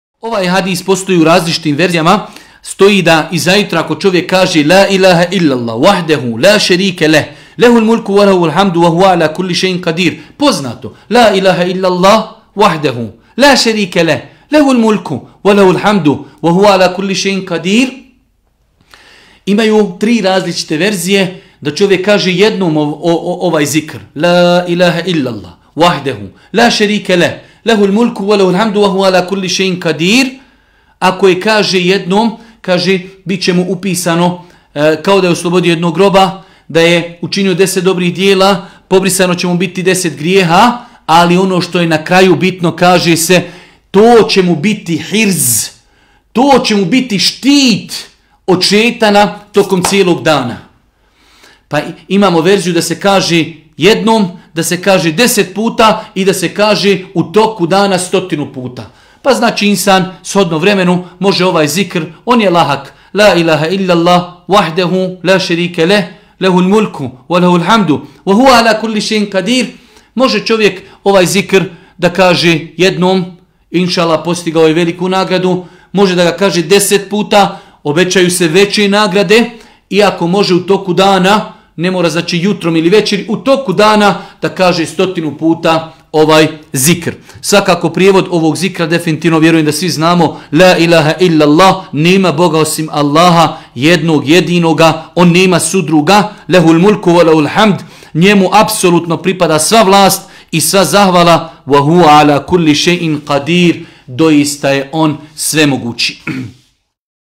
JUTARNJI I VEČERNIJI ZIKR 8.DIO - ZAŠTITA OD ŠEJTANA CIJELI DAN 2020
“La ilahe illellahu vahdehu la šerike lehu, lehulmulku ve lehulhamdu ve huve ‘ala kulli šej‘in kadir”

– Kazao je Allahov Poslanik, sallallahu `alejhi we sellem: 'Svako ko, čim ujutro ustane, izgovori dovu –

“Nema boga osim Allaha, Jedinoga, Koji druga nema. Njemu pripada sva vlast i zahvalnost i On je svemogući” – imat će nagradu u vrijednosti onoga ko oslobodi roba iz Ismailovog, a.s., potomstva, bit će mu pobrisano deset grijeha, bit će uzdignut za deset stupnjeva, zaštićen od šejtana sve do noći. A kada u noć uđe i prouči isto, slijedi mu to isto sve do sabaha.'